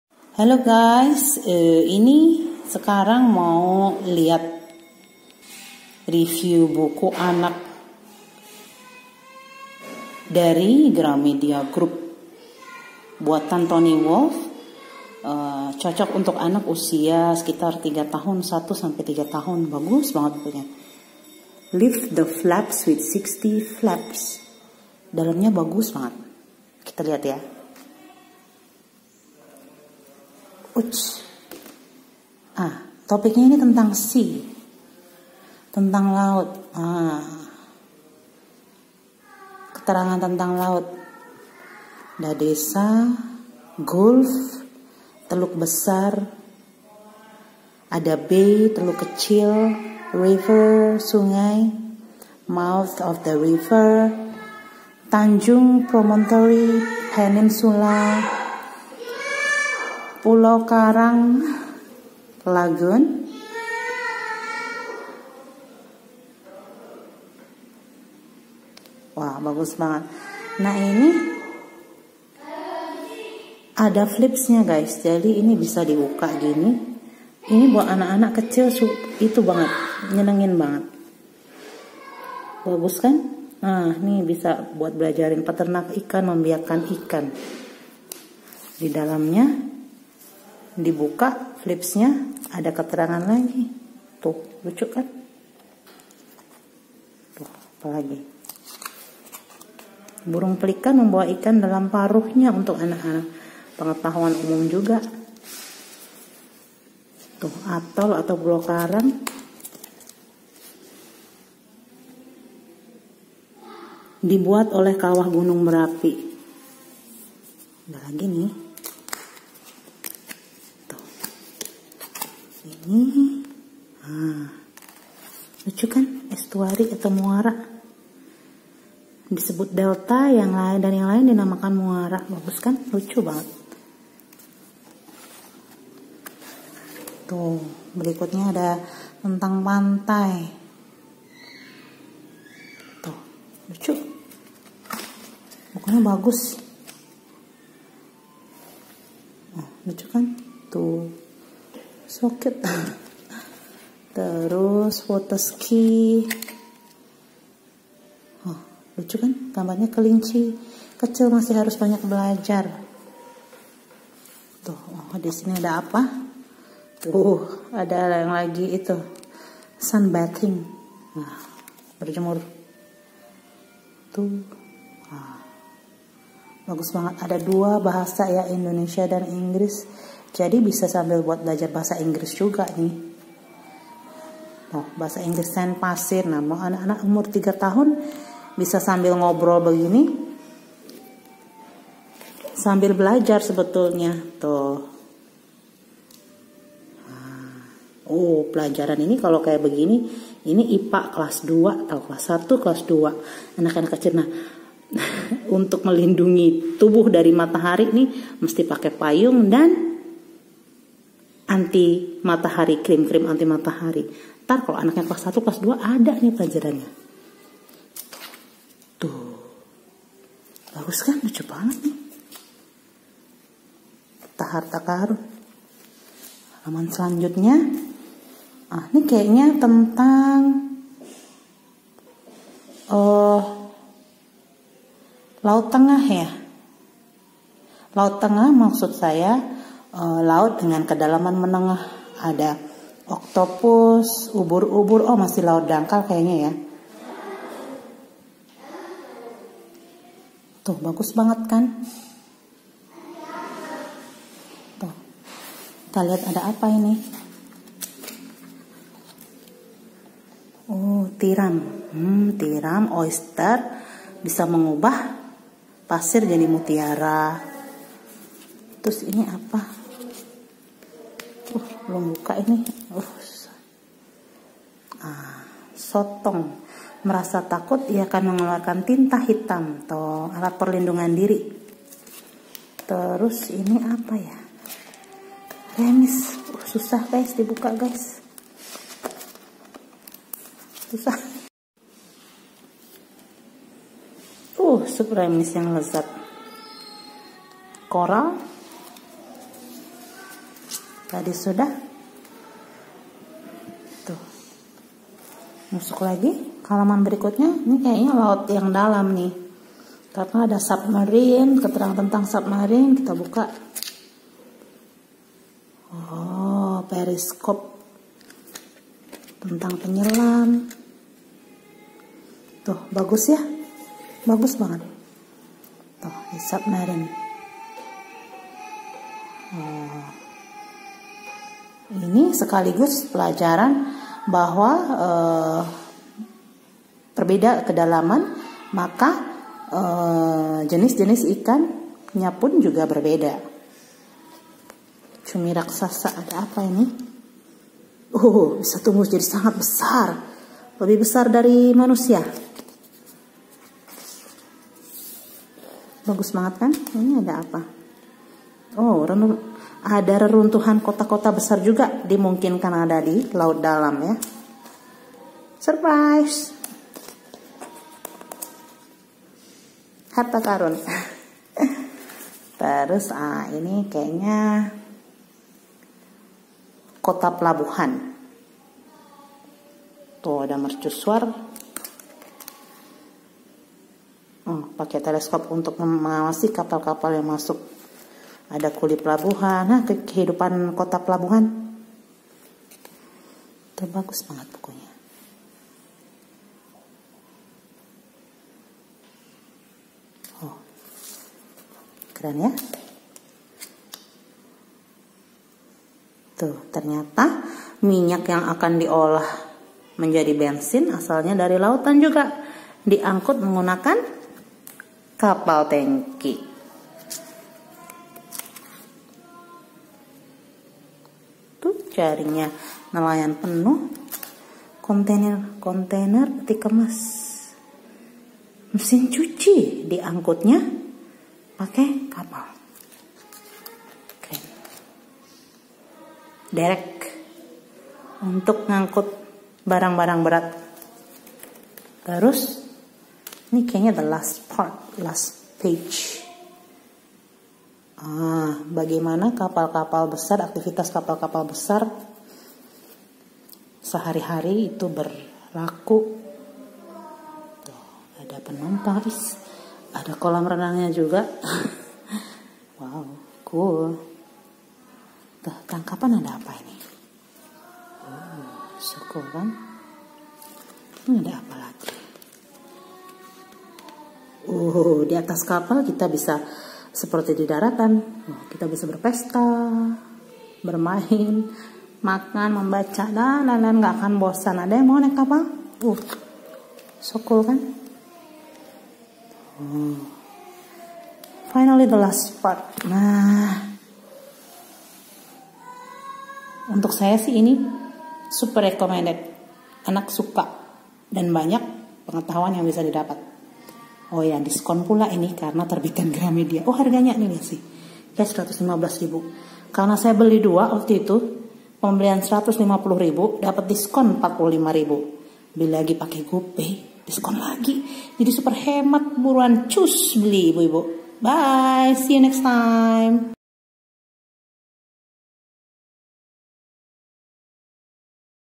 Review buku anak terbitan Gramedia: Lift the flaps with 60 flaps seri LAUTAN
Review buku anak cocok untuk usia 3-5tahun.  Ada info harga di last part.  Tonton yaaa bu